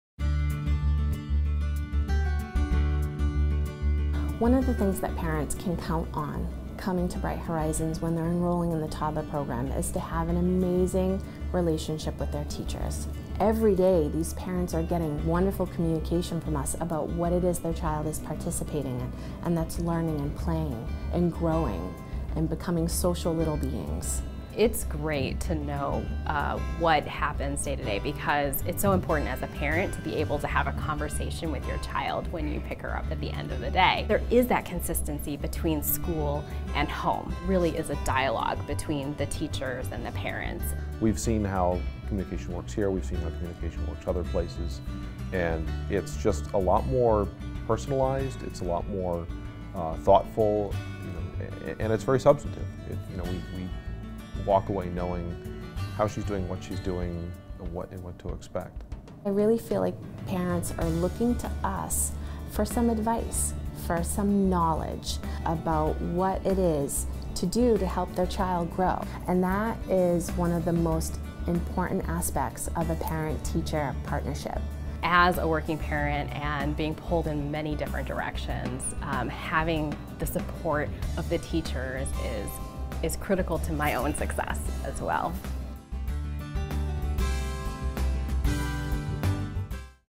Toddler Teachers at Bright Horizons
At Bright Horizons, parents can count on an amazing relationship with their child’s teachers. Hear how these strong partnerships make communication easier, keep parents informed, and allow for consistency between school and home.